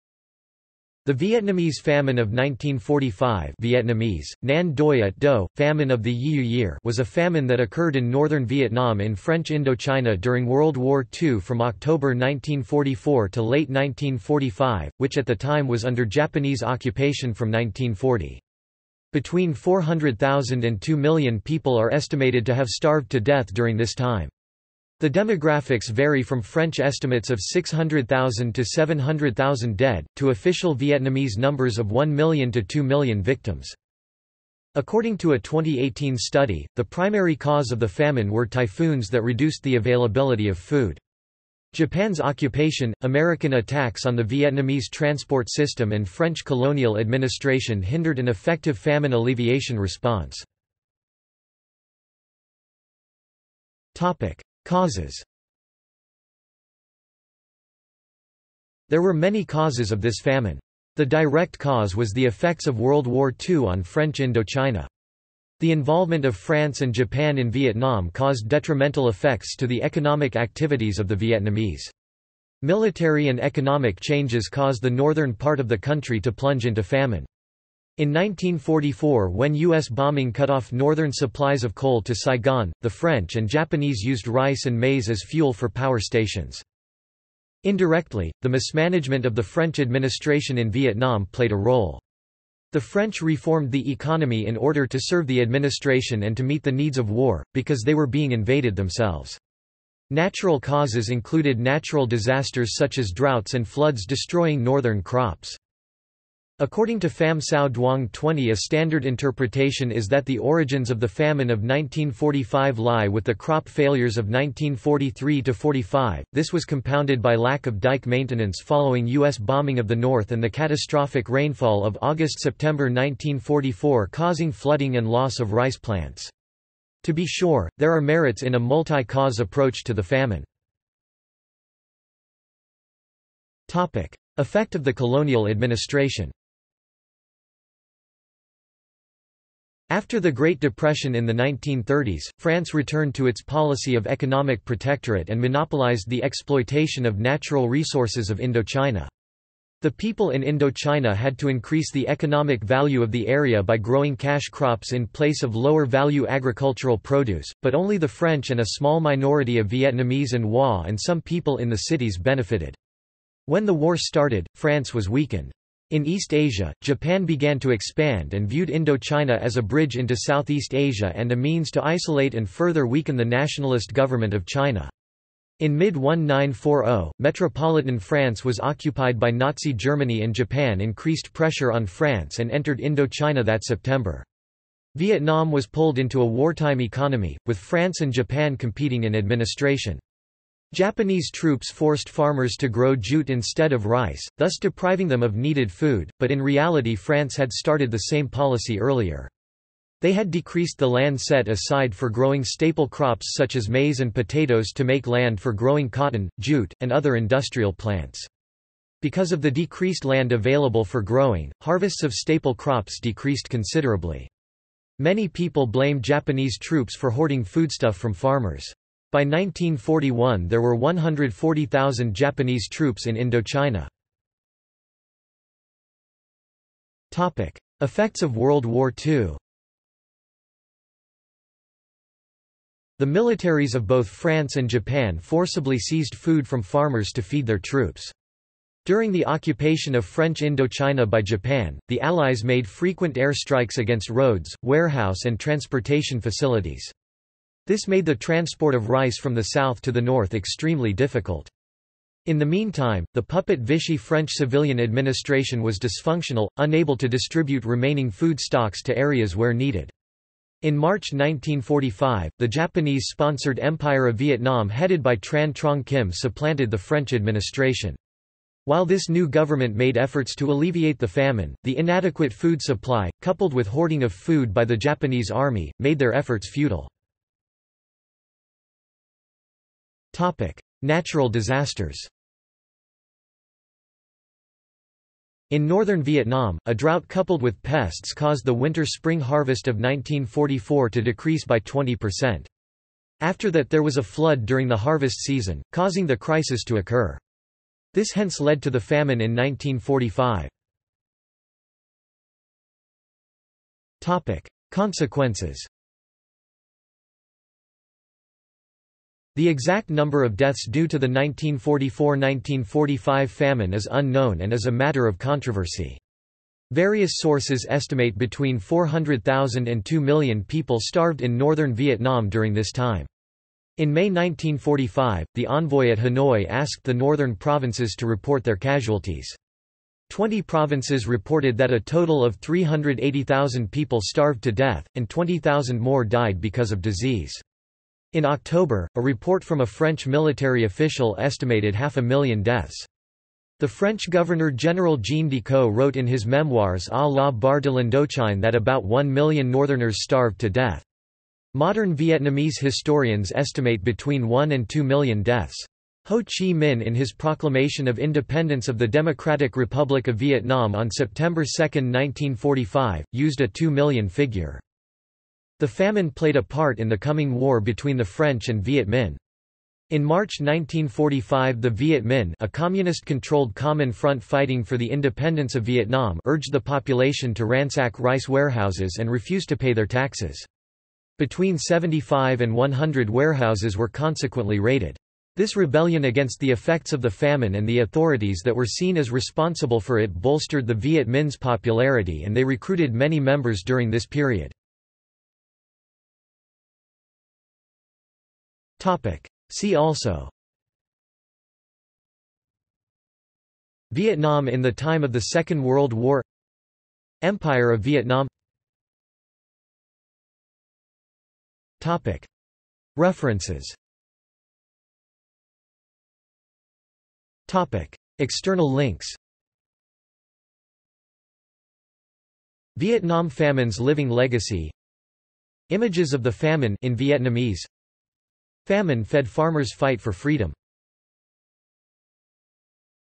Vietnamese Famine of 1945 | Wikipedia audio article
This is an audio version of the Wikipedia Article:
Vietnamese Famine of 1945

- Socrates

SUMMARY
The Vietnamese Famine of 1945 (Vietnamese: Nạn đói Ất Dậu - Famine of the Yiyou Year) was a famine that occurred in northern Vietnam in French Indochina during World War II from October 1944 to late 1945, which at the time was under Japanese occupation from 1940. Between 400,000 and 2 million people are estimated to have starved to death during this time. The demographics vary from French estimates of 600,000-700,000 dead, to official Vietnamese numbers of 1,000,000 to 2,000,000 victims.
According to a 2018 study, the primary cause of the famine were typhoons that reduced the availability of food. Japan's occupation, American attacks on the Vietnamese transport system and French colonial administration hindered an effective famine alleviation response.